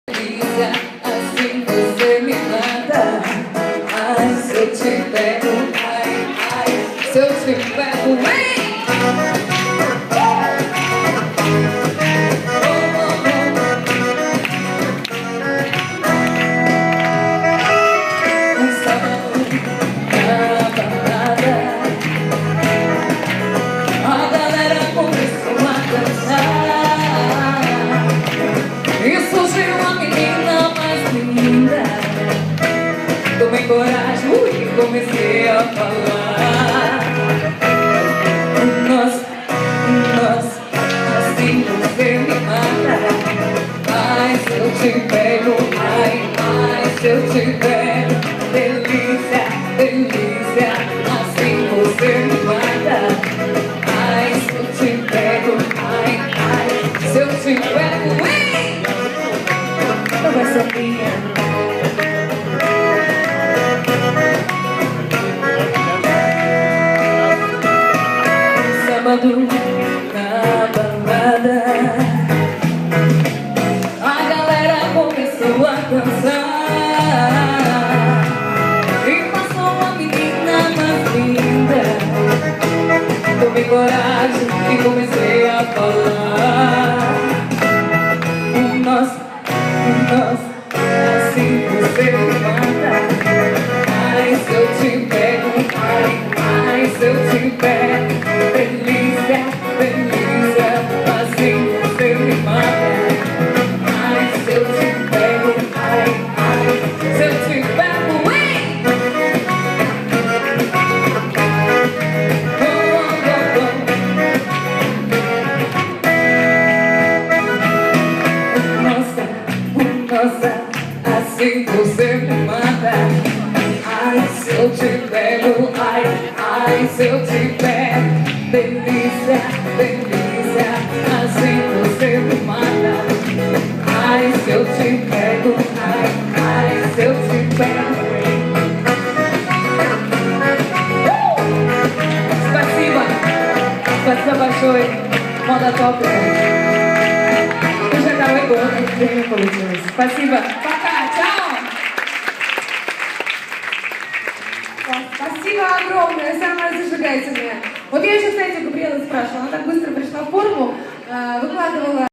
Премия "Мама года 2014" Gabriella da Silva
26 июня в Детской Галерее "Якиманка" прошла IV ежегодная премия "Мама года", организованная журналом MODA topica